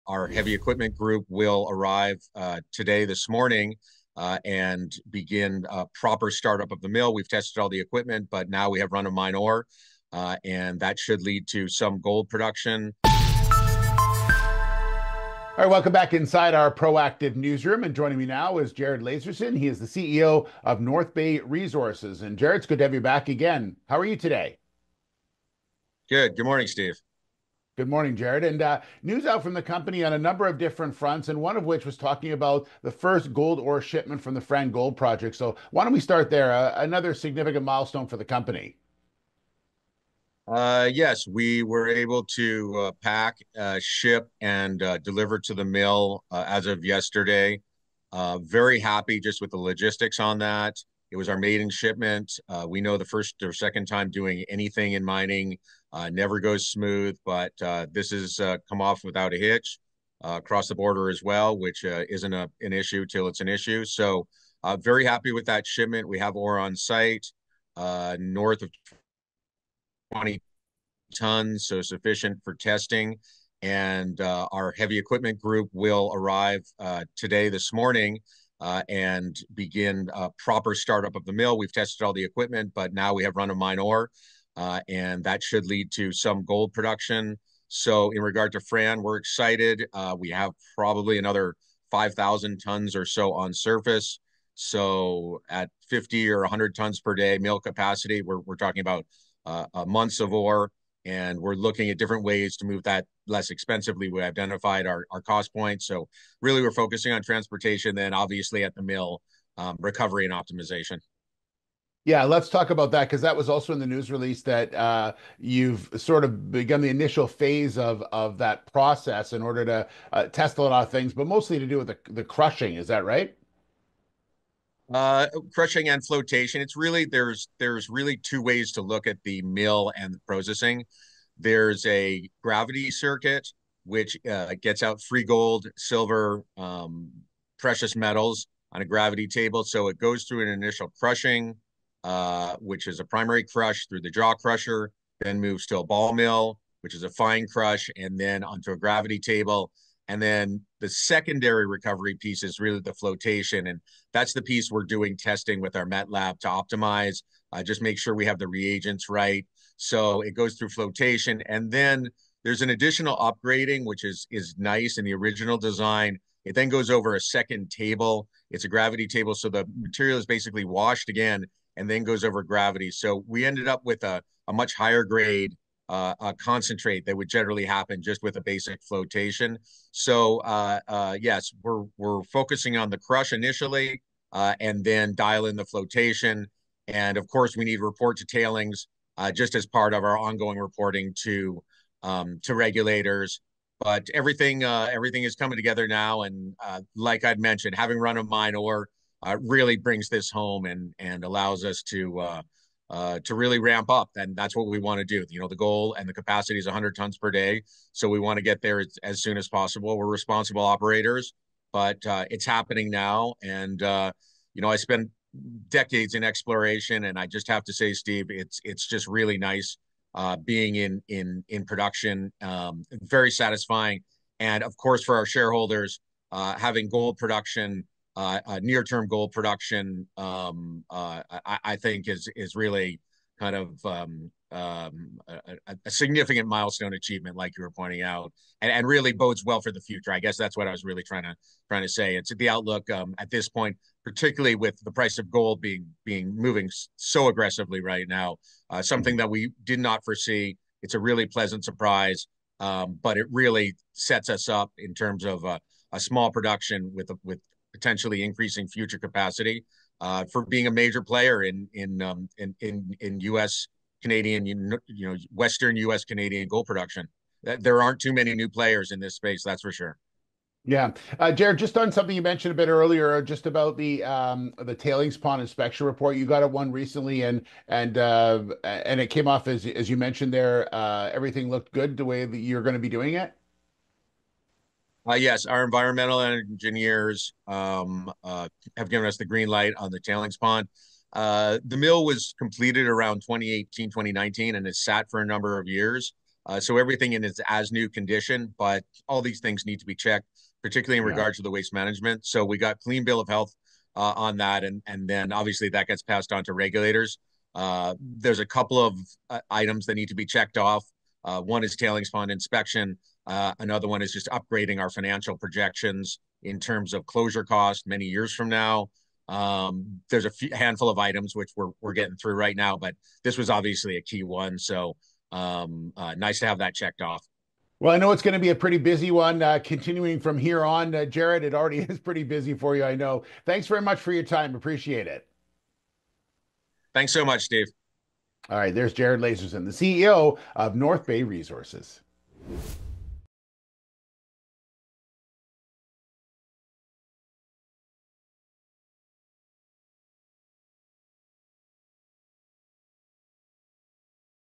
North Bay Resources Begins Test Milling at Fran Gold Project with First Gold Ore Shipment
North Bay Resources CEO Jared Lazerson joined Steve Darling from Proactive to announce the commencement of test milling operations following the arrival of the first gold ore shipment from the Fran Gold Project. This marks a significant step forward for the company as it begins the testing and optimization of its milling process to extract gold from the ore.

The initial phase of the milling process will focus on testing the jaw crusher to reduce the ore size to minus ½ inch, making it suitable for feeding into the ball mill. The ball mill will further reduce the ore to minus 150 mesh, after which the material will be converted into a slurry and moved to the gravity separation table. This final step aims to extract free gold and other valuable metals from the ore.

In addition to the milling updates, Lazerson shared that North Bay Resources has received a tailings pond inspection report from its environmental engineering firm. The report confirmed that there are no issues or degradation of the tailings pond liner at the Bishop Gold Mill’s Waste Management Unit. The company is also working on a De-watering Plan and updated Waste Management and Closure financial projections with its civil and process engineering firm. These documents will be used to assess whether the current $250,000 closure bond requirement will need adjustment by regulators.

With these critical steps underway, North Bay Resources is making steady progress in advancing its Fran Gold Project towards full-scale production while ensuring compliance with environmental standards.